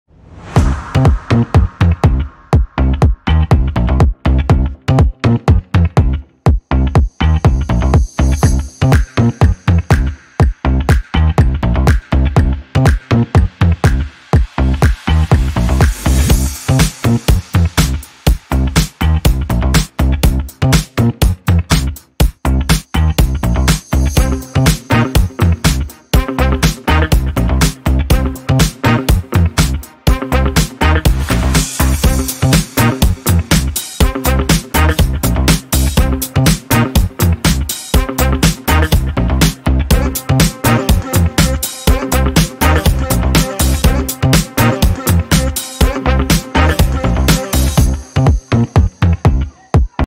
Citroën C4 wrc 2007 1/43
WRC project No31
World Rally Champion in 2007
Winner: Sebastian Loeb (driver) Daniel Elena (co-driver)
Original model: Ixo 1/43
Skin: S.Loeb - D.Elena
      75. Rally Automobile
             Monte Carlo 2007 winner
         and 2007 World Champion